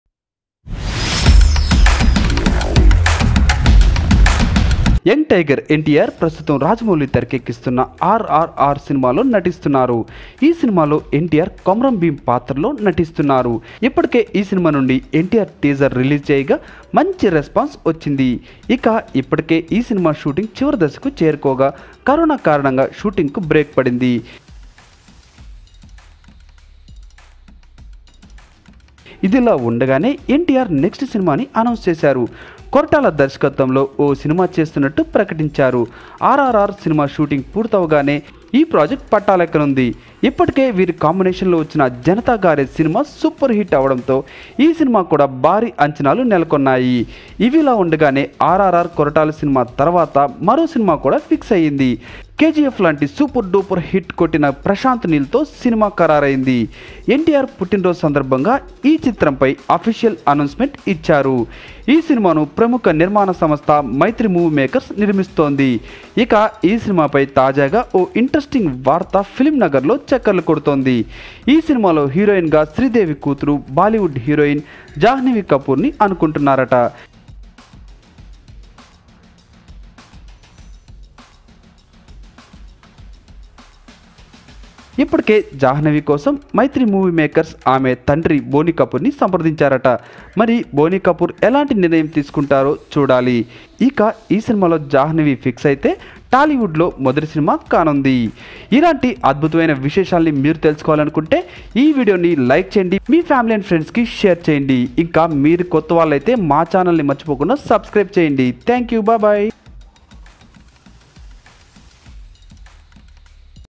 ఎన్టీఆర్ తో శ్రీదేవి కూతురు రొమాన్స్.! | NTR Jhanvi Kapoor | NTR Prashanth Neel Movie | Cinemaizm
Actor Janhvi Kapoor may be seen making her Telugu debut. According to a report, she may be paired with Jr NTR in a film by Trivikram.

ఎన్టీఆర్ తో శ్రీదేవి కూతురు రొమాన్స్.! | NTR Jhanvi Kapoor | NTR Prashanth Neel Movie